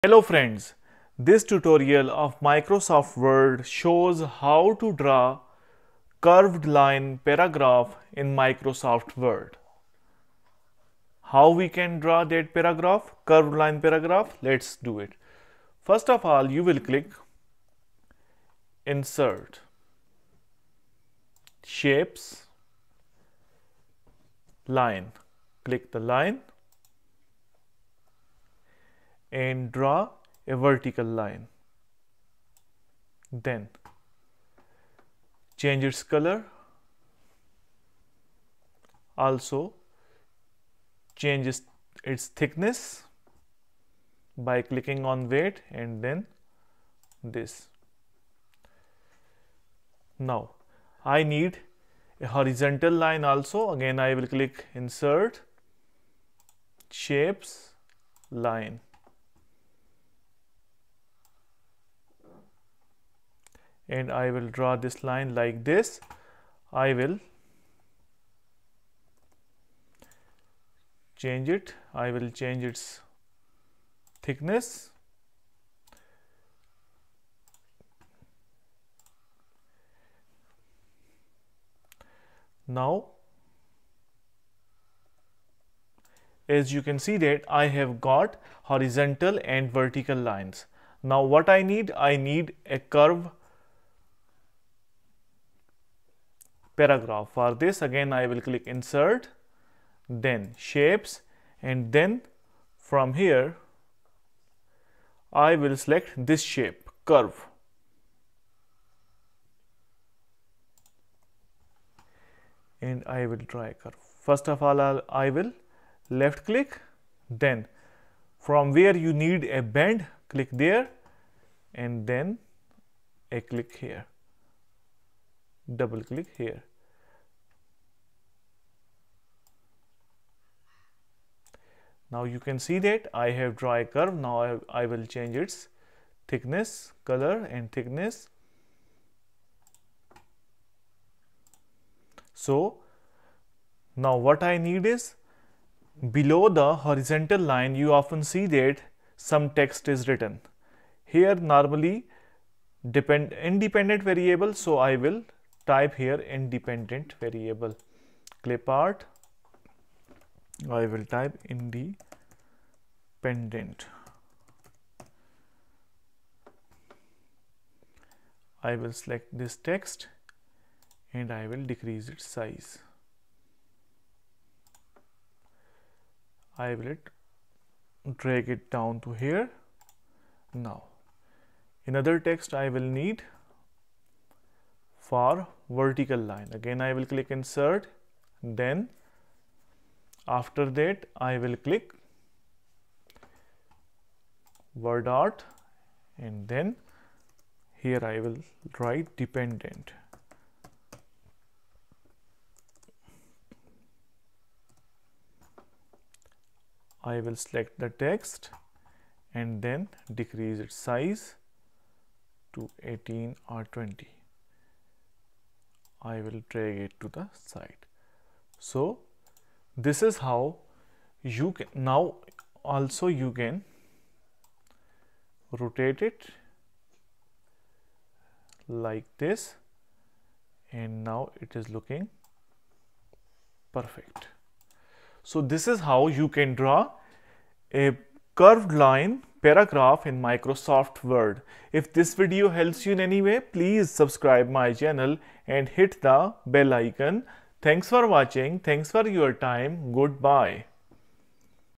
How To Draw Curved Line Graph In Microsoft Word | Make Curve Graph In MS Word
this Microsoft Word tutorial shows how to draw a curved line paragraph in Microsoft word. in many subjects you need to draw curved lines so here  it is the tutorial for drawing the curved lines in Microsoft word. This tutorial will work in Microsoft Word 2007,2010, 2013, 2016 in 2020